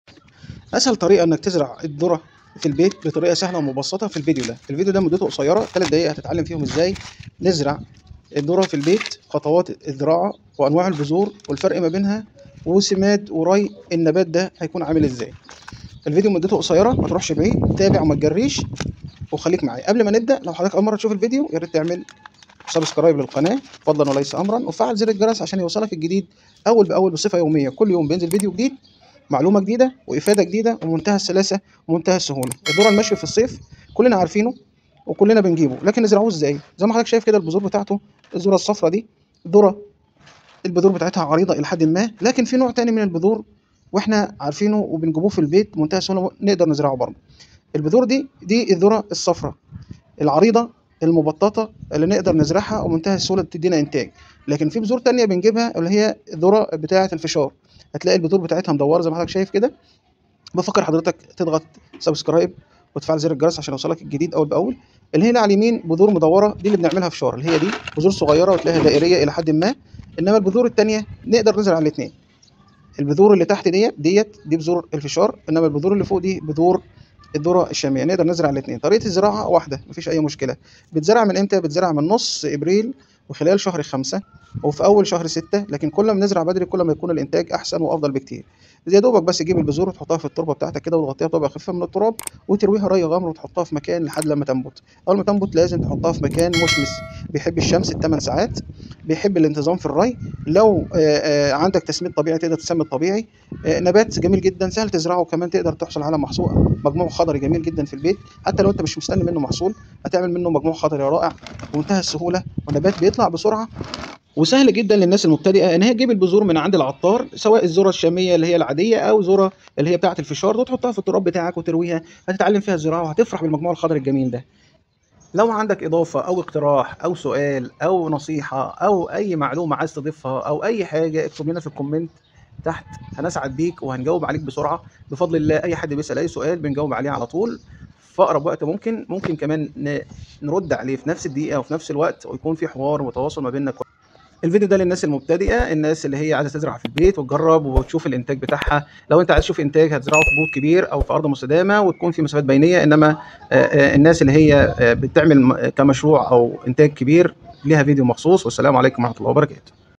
كيف تزرع طن من الذرة الحلوة فى المنزل كيفية زراعة الذرة للمبتدئينGrowing Corn in the yard house
زراعة الذرة على الشرفة ، كيفية زراعة الذرة في المنزل للمبتدئين إذا كنت ترغب في معرفة كيفية زراعة محصول كبير من الذرة الحلوة  زراعة الذرة في المنزل أمر بسيط ، قم بزراعة الذرة الحلوة من البذور
 Growing Corn in the yard house and harvesting
5 نصائح حول كيفية زراعة طن من الذرة الحلوة في اناء أو حاوية حديقة مرتفعة